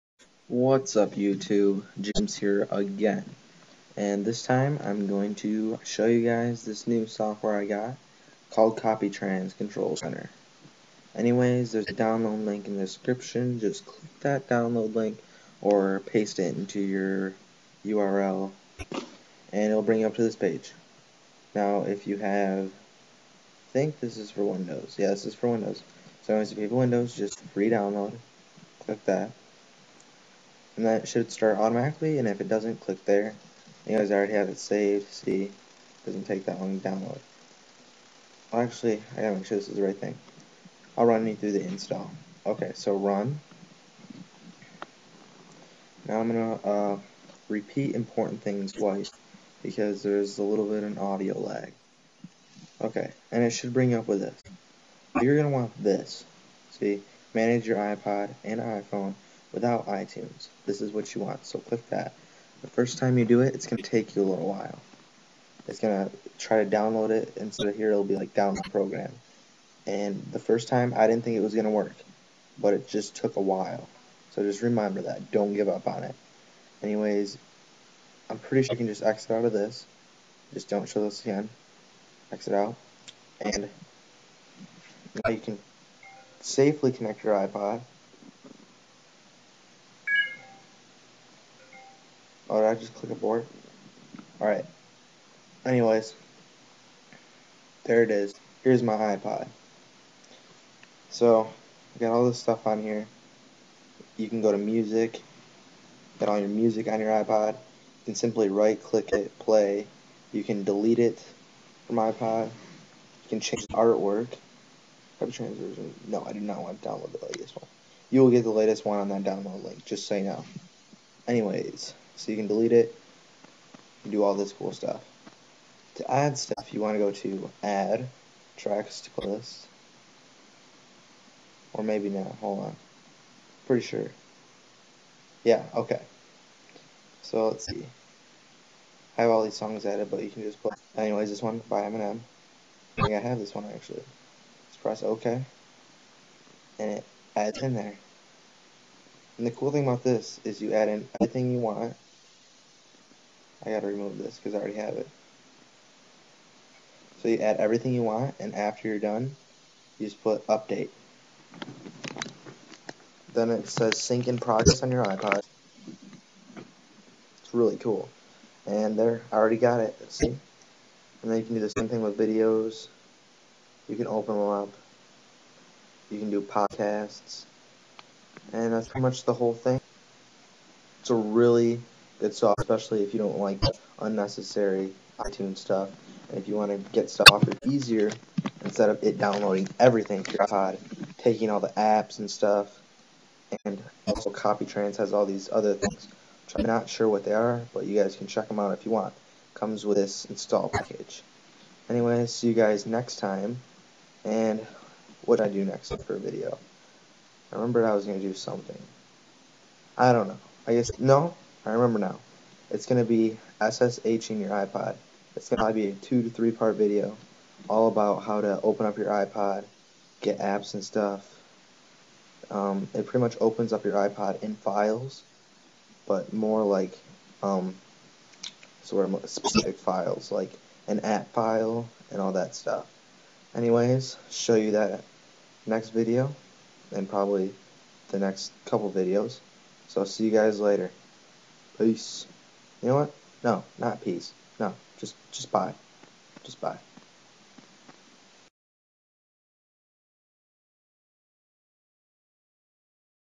Manage Ipod/Iphone without itunes! With Commentary
This software is called copy trans

I did not create copy trans just go for more infromation